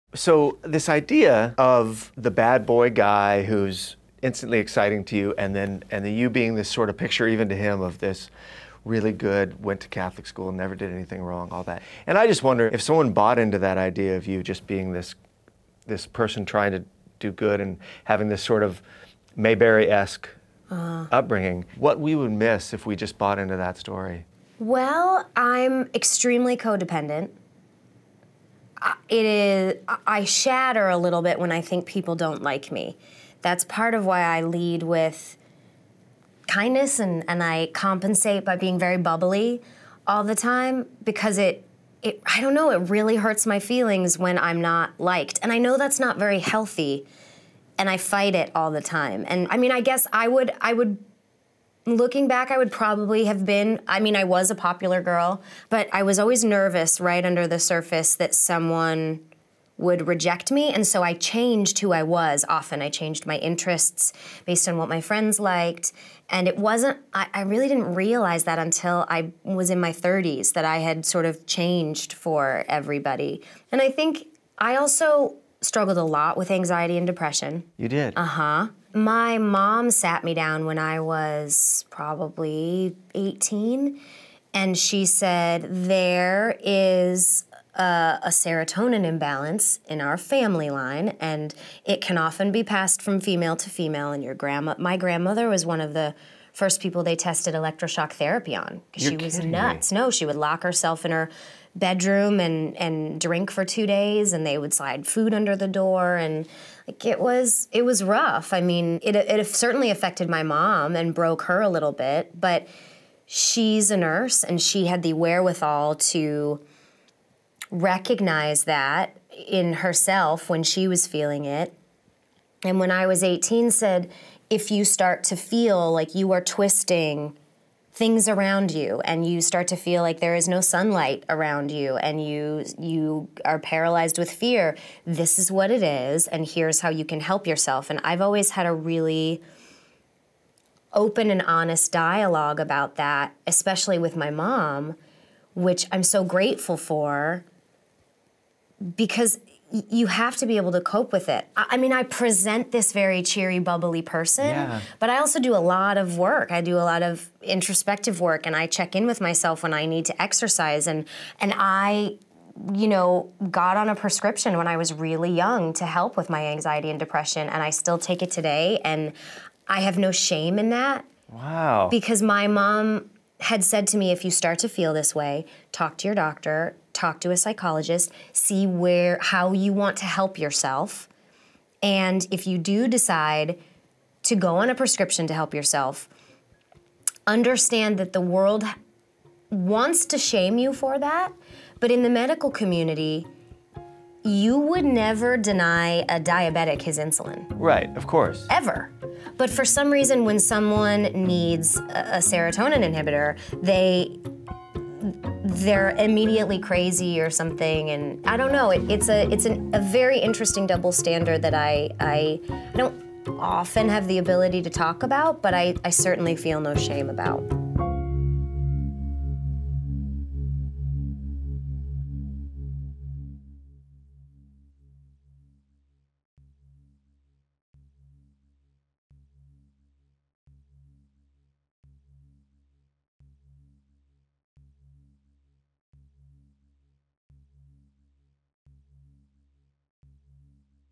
Kristen Bell Explains There Is No Shame In Feeling Anxiety & Depression
If you liked this video, there's a lot more where it came from. This is a small excerpt from a much longer conversation that you can watch right